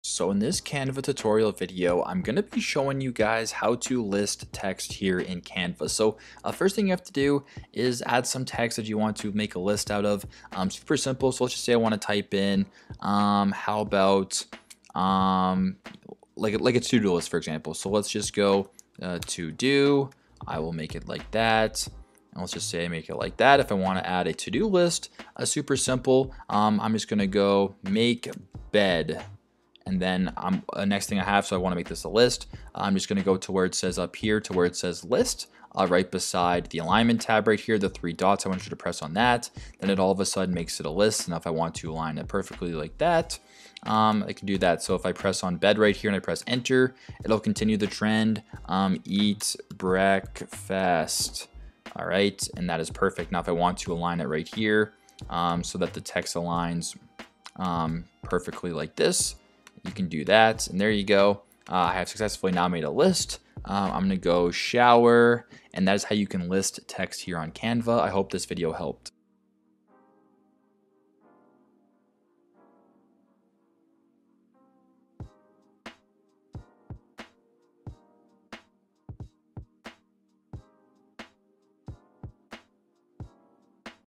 How To List Text In Canva | Canva Tutorial
To create a list in Canva, you can follow these simple steps:

Select the text box where you want to create a list by clicking on it.

You should see a bounding box around the text.

Type in the text you want to include in the list, and hit "Enter" after each item to create a new line.

Highlight the first item in the list by clicking and dragging over it.

In the top toolbar, click on the "Bullets" icon to apply bullet points to the selected item.

You can choose from a variety of bullet point styles. Repeat step 4 for each item in the list.

If you want to create a numbered list instead of a bulleted list, click on the "Numbers" icon in the top toolbar instead of the "Bullets" icon.

You can further adjust the font style, size, and color from the same toolbar.

Once you're done formatting your list, you can click outside the text box to save your changes.

That's it! You have now learned how to create a list in Canva